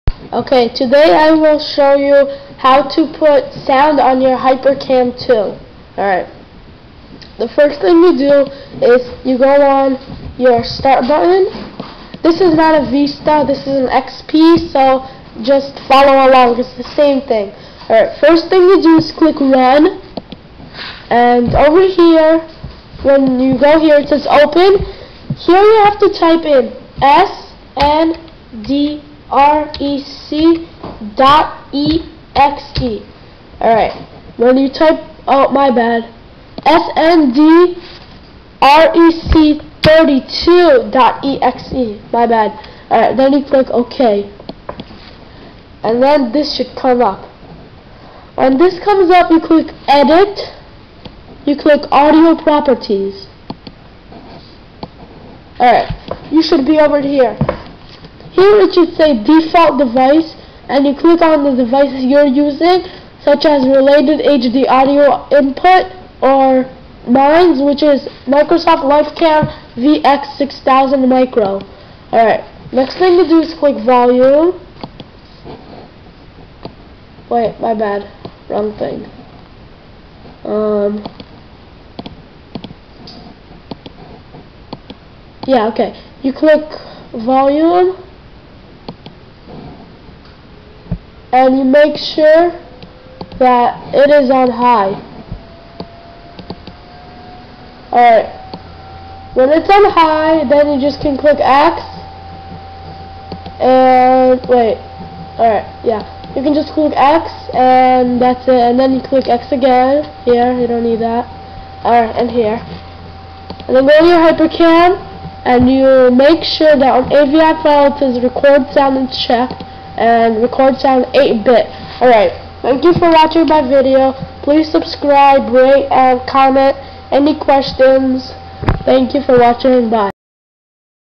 how to put sound on hypercam 2
how to put sound on hy cam 2 using sndrec32.exe  related hd audio input microsoft lifecam vx 6000 micro